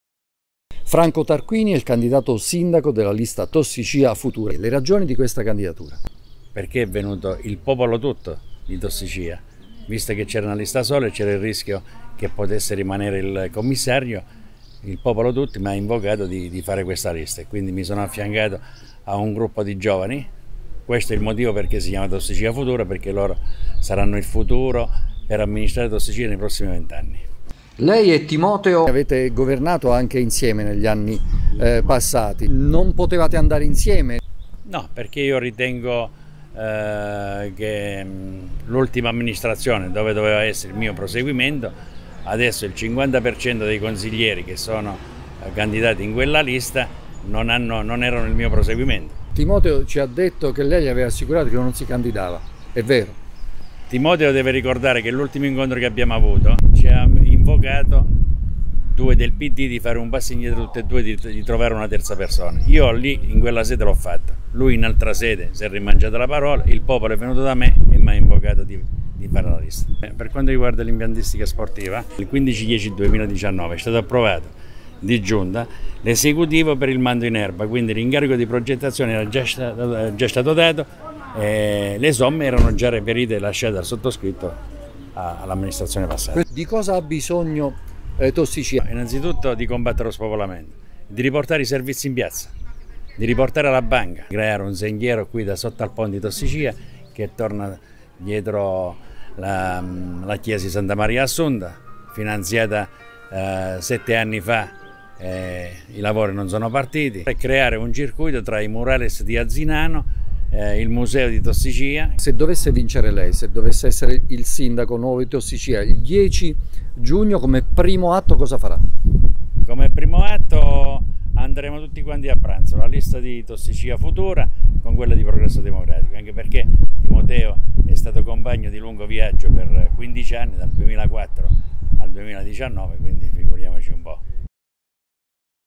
Tossicia | Franco Tarquini già sindaco del paese alla guida della lista civica "Tossicia Futura"
| SUPER J |

Contatti Redazione:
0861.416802